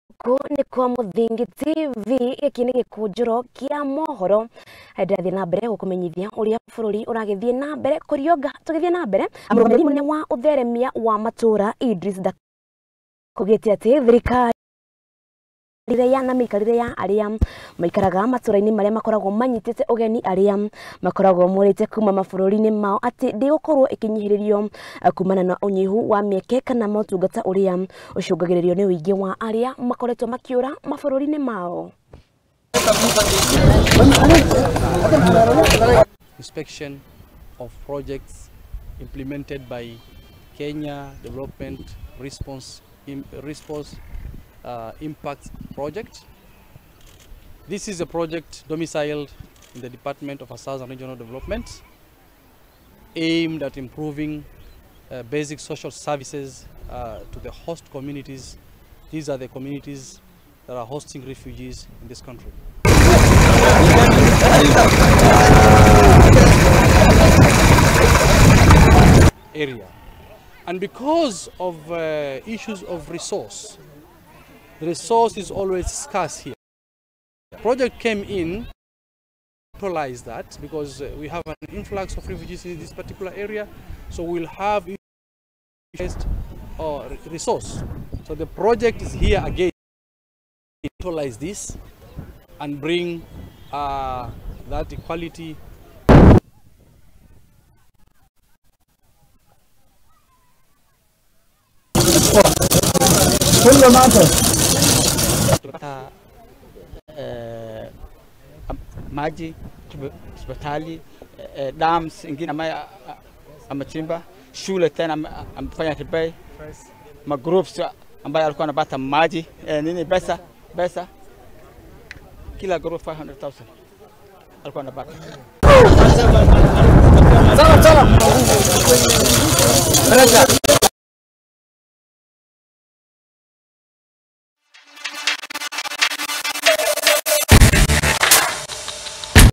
GUTHAITARA ETHARI NGUI.
Murugamiriri munene wa utheremia wa matuura  idris dakota ni augiite ati thirikari ni igutigirira miturire na miikarire ya aria maikaraga matuura maria makoragwo manyitete ugeni aria makoragwo moorite kuuma mabururi mao ati gutigokorwo na kuhinyiririo kuumana na unyihu wa mieke kana mautungata uria ucungagiririo ni uingi wa aria moorite mabururi mao